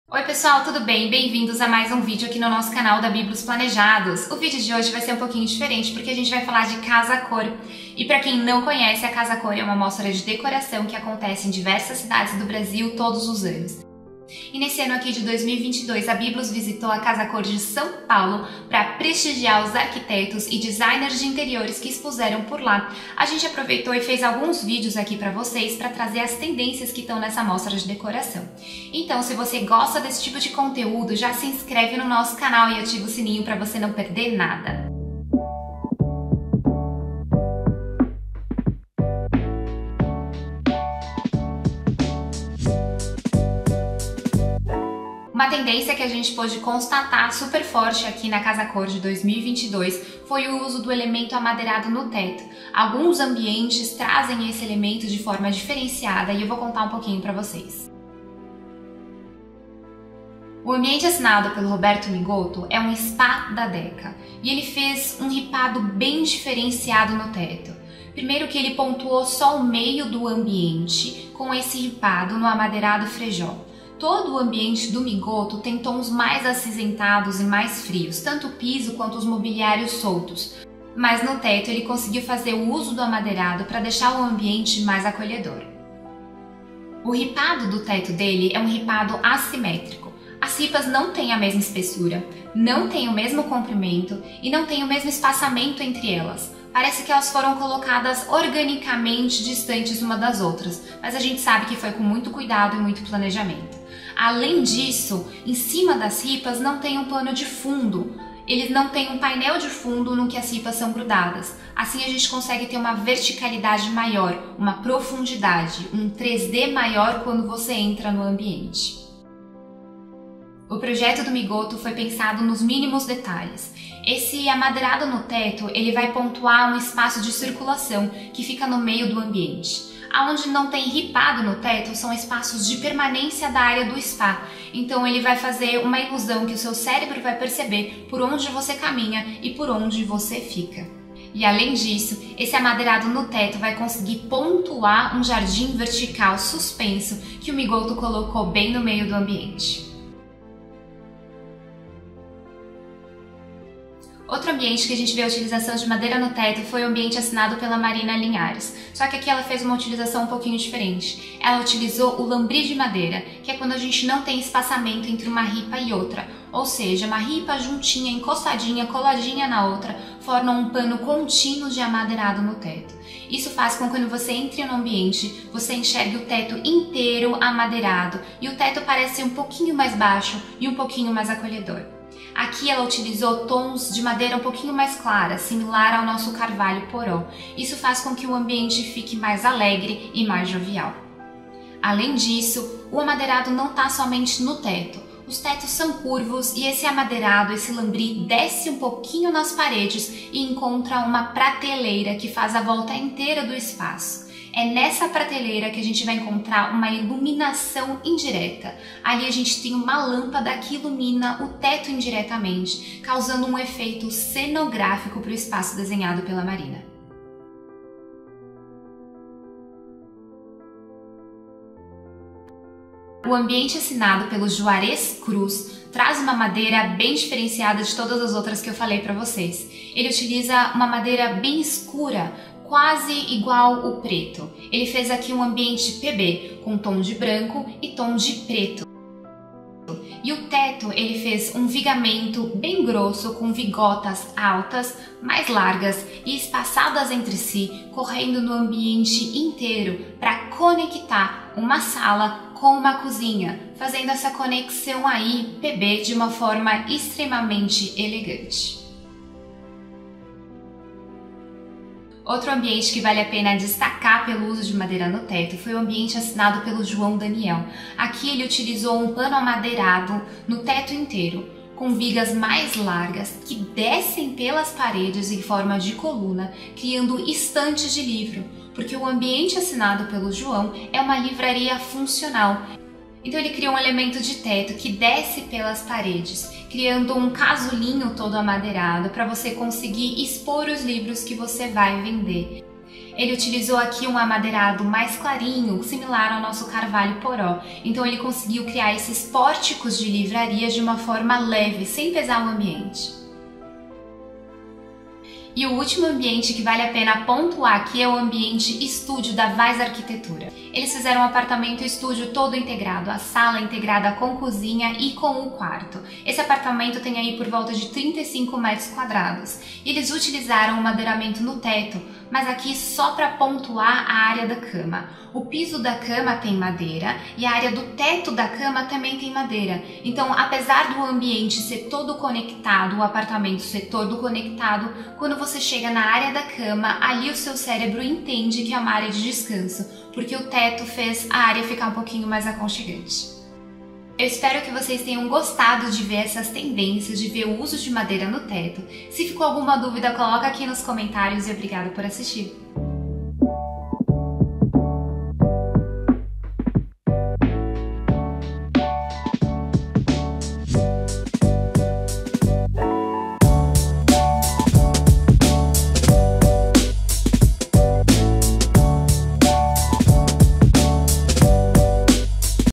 Tendências de decoração na Casa Cor São Paulo em 2022 , Tetos amadeirados por Biblos Planejados
Hoje vamos falar para vocês de uma tendência de decoração marcante , que vimos na casa cor de São Paulo de 2022.
Tetos com aplicação de madeira.

Desde tetos ripados, até tetos com lambri de madeira , vamos te mostrar nesse video os ambientes dos profissionais que utilizaram esse recurso de formas lindas , elegantes e super interessantes .

Os ambientes do video foram desenhados por profissionais arquitetos e designers de interiores e aqui é a nossa pequena homenagem a eles .